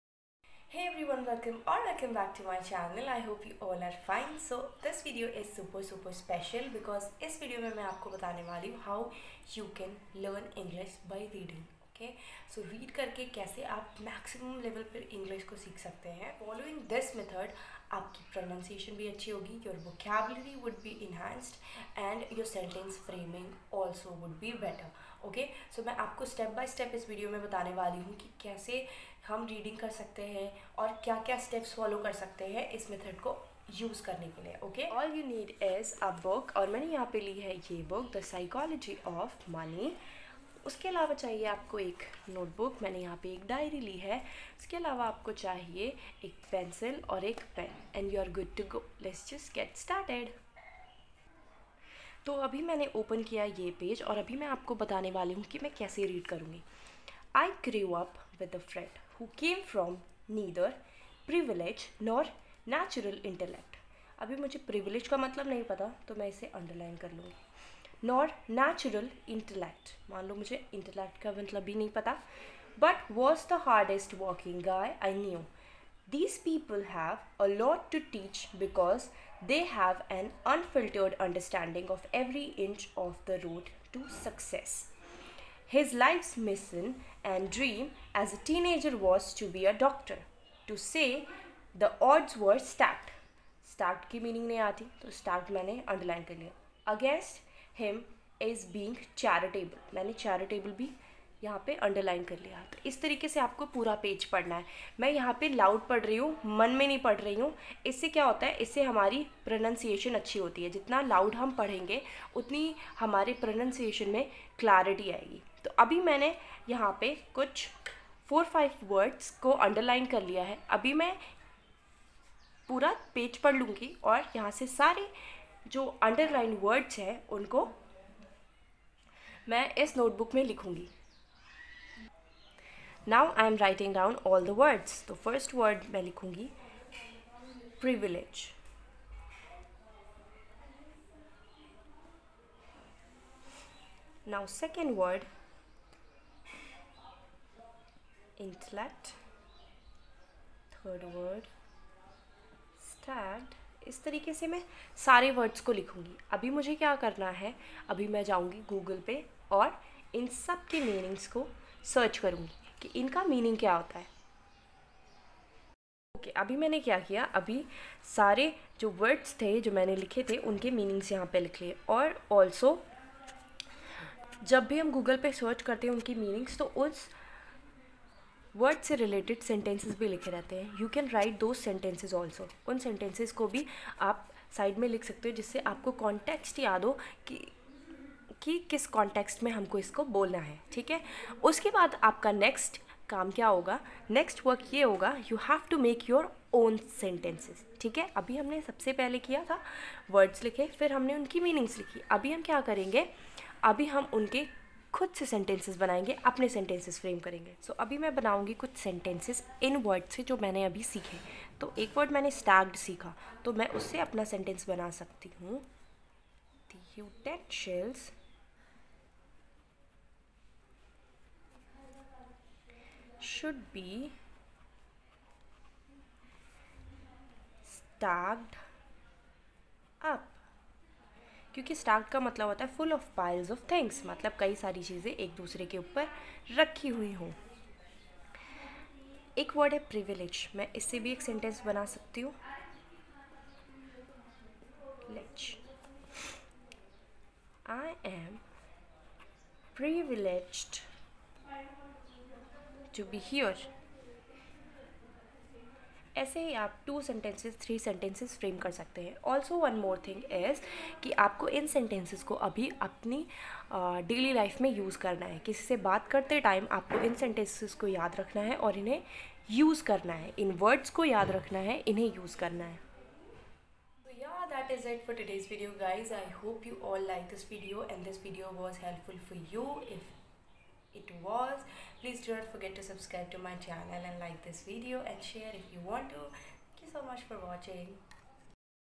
How to improve English by Reading
Hey guys!
In this video I have shared how you can read correctly to improve your English